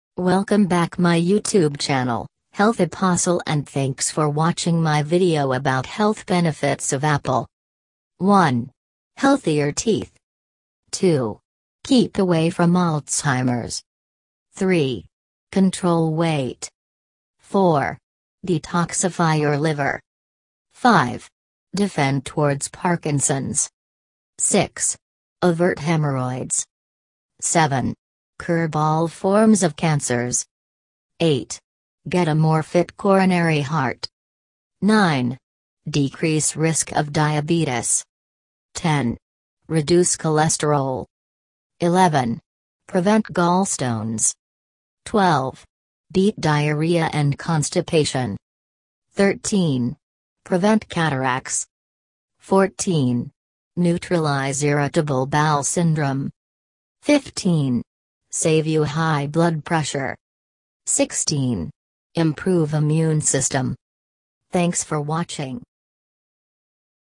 Benefits of Apple
Health Benefits of Apple

1. Healthier Teeth
2. Keep away from Alzheimer’s
3. Control weight
4. Detoxify your liver
5. Defend towards Parkinson’s
6. Avert hemorrhoids
7. Curb all forms of cancers
8. Get a more fit coronary heart
9. Decrease risk of diabetes
10. Reduce cholesterol
11. Prevent gallstones
12. Beat diarrhea and constipation
13. Prevent cataracts
14. Neutralize irritable bowel syndrome
15. Save you high blood pressure
16. Improve immune system